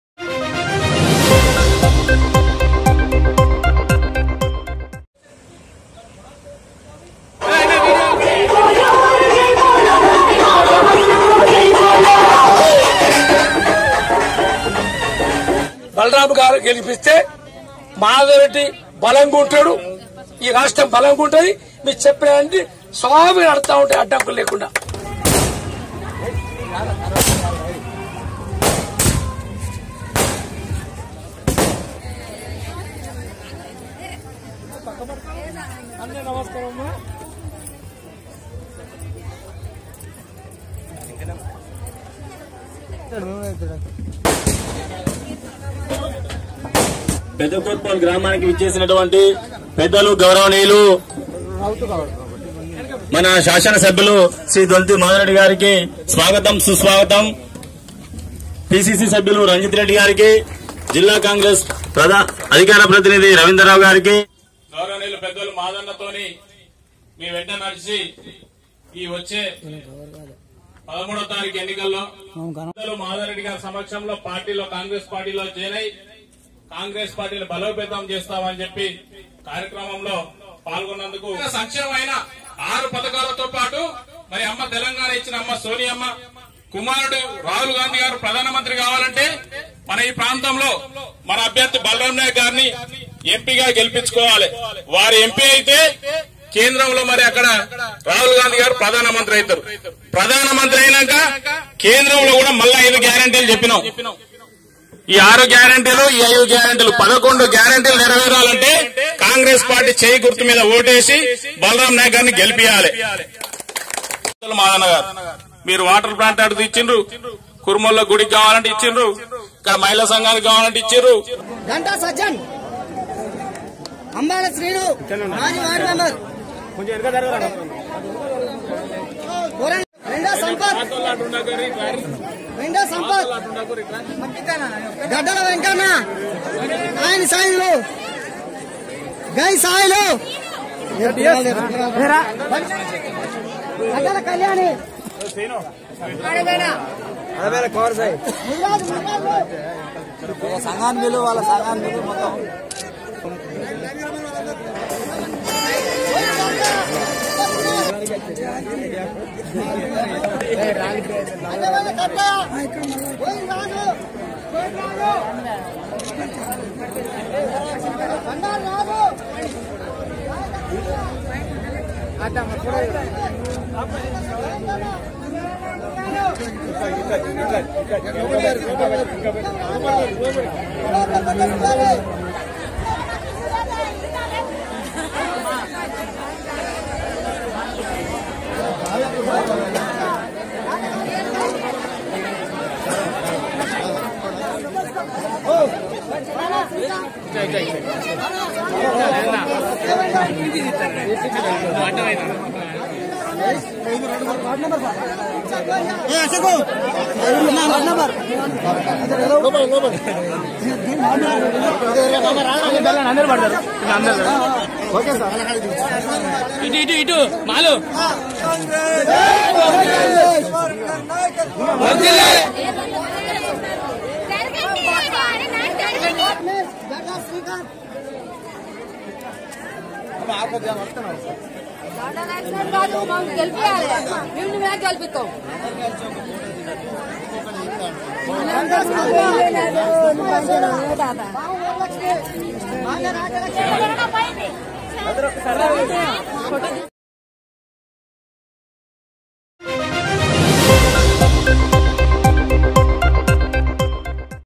08/05/2024 BRS Nundi Congress lo Bhaari Cherikalu.
Nekkonda Mandalam lo BRS Khaali